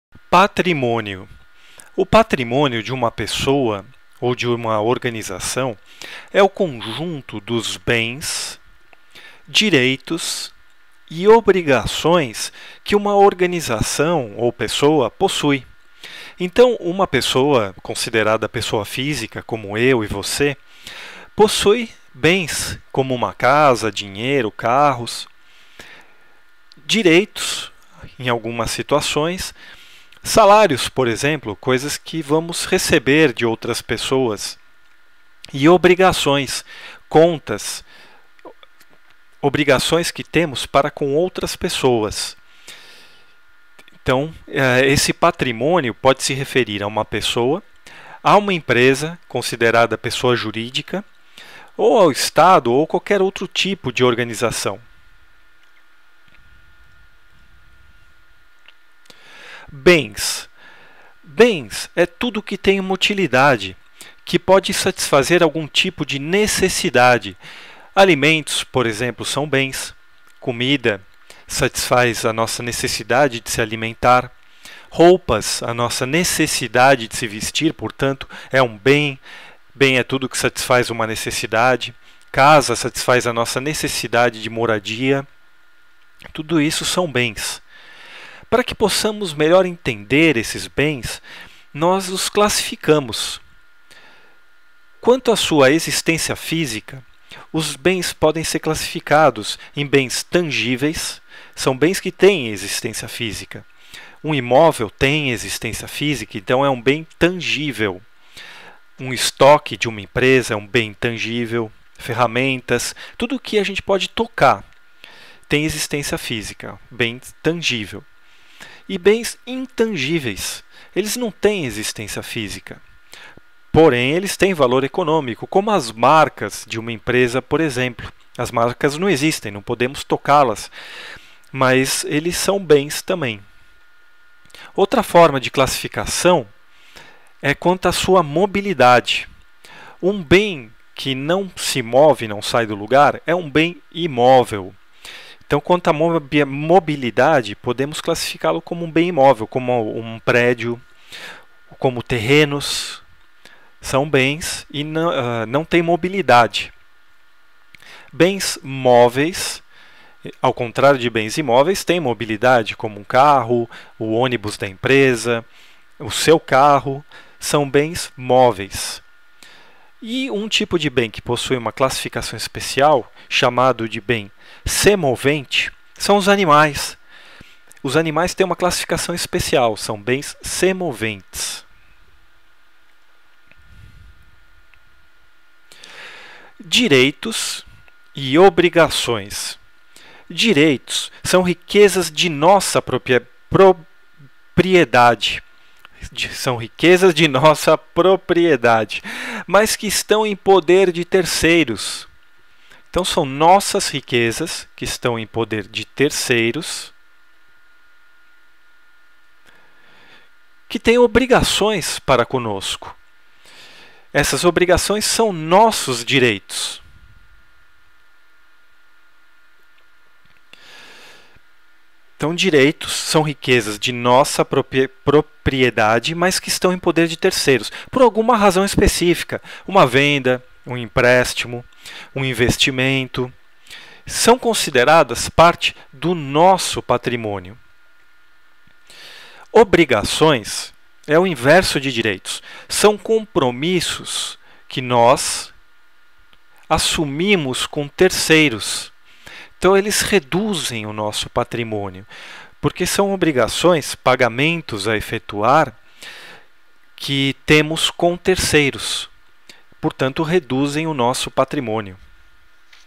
Patrimônio, bens, direitos e obrigações CONTABILIDADE 1.2
O patrimônio de uma pessoa ou empresa é o conjunto dos bens, direitos e obrigações que uma organização ou pessoa possui.
Um bem é tudo que tem utilidade, coisas que podem satisfazer algum tipo de necessidade. Os bens podem ser divididos em algumas classificações específicas: tangíveis, intangíveis, móveis, imóveis e semoventes.
Direitos e obrigações.

Autor: Mário Attab
AVENIDACULT The CULT channel

(O conteúdo deste vídeo e respectivo curso visa meramente oferecer material para estudo autodidata, sem relação com formação de qualquer natureza e não substitui ou dispensa a consulta a um profissional qualificado.
A prática da profissão contábil depende de registro no Conselho Regional de Contabilidade.